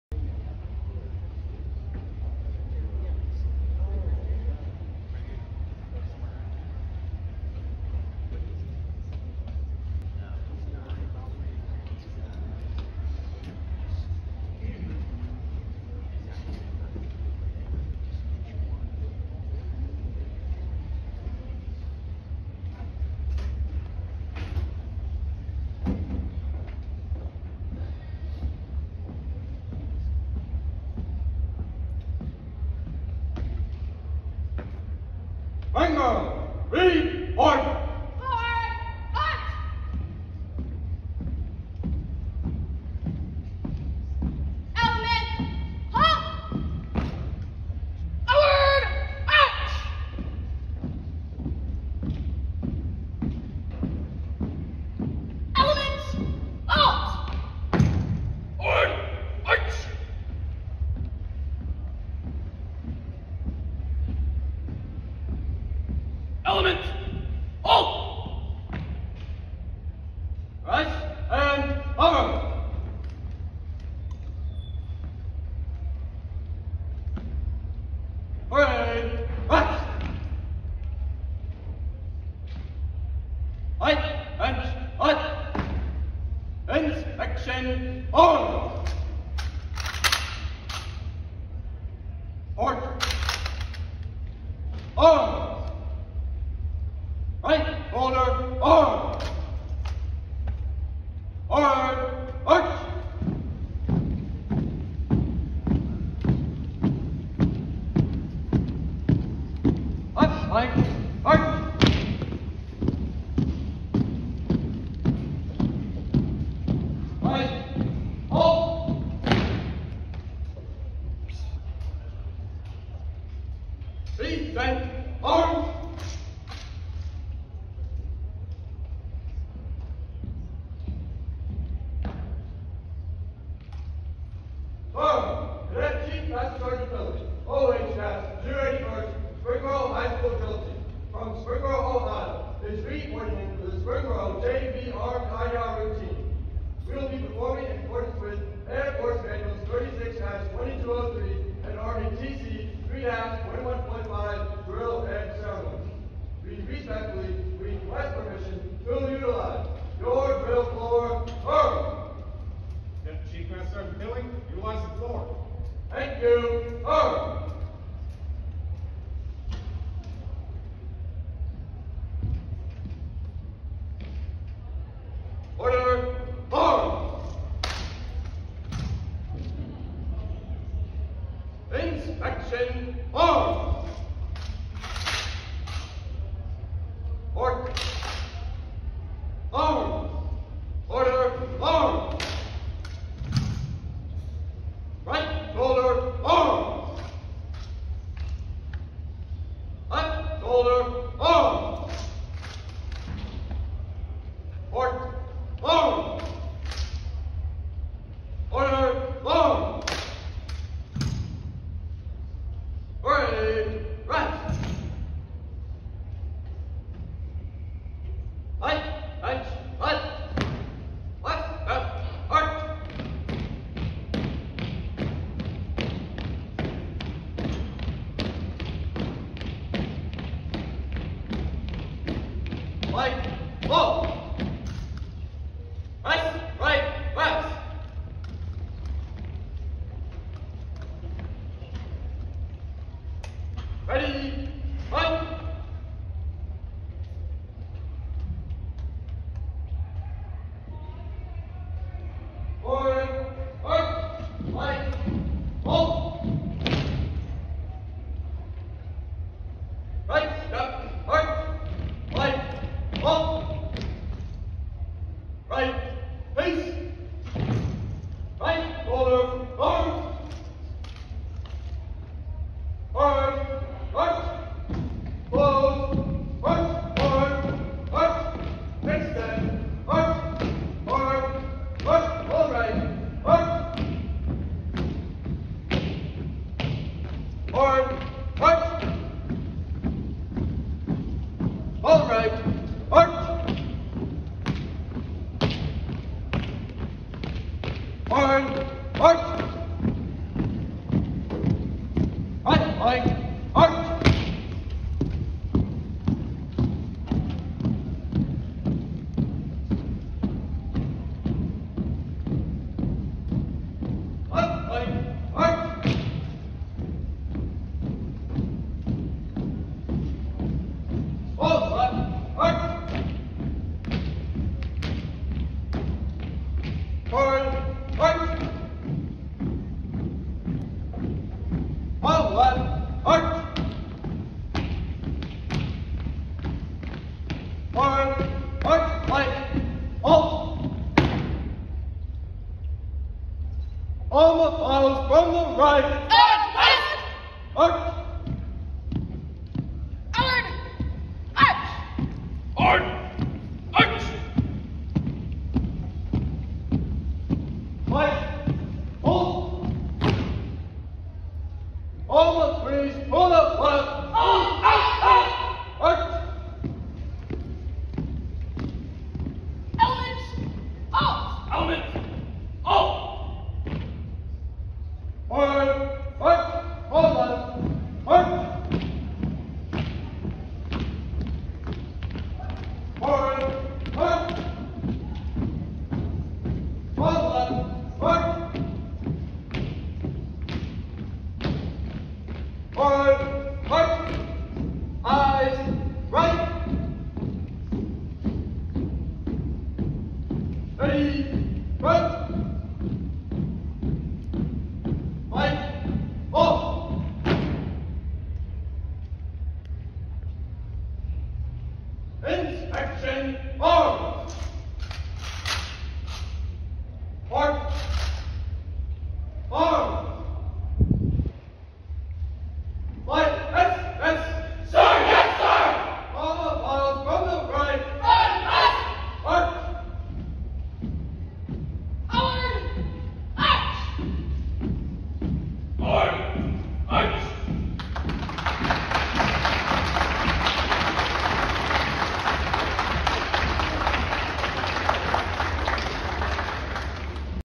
Walter E. Stebbins Memorial JROTC -- Drill Team Championships - Springboro JV Armed - 5 Mar 2022
Springboro JROTC Junior Varsity Armed performance captured at the meet on 5 Mar 2022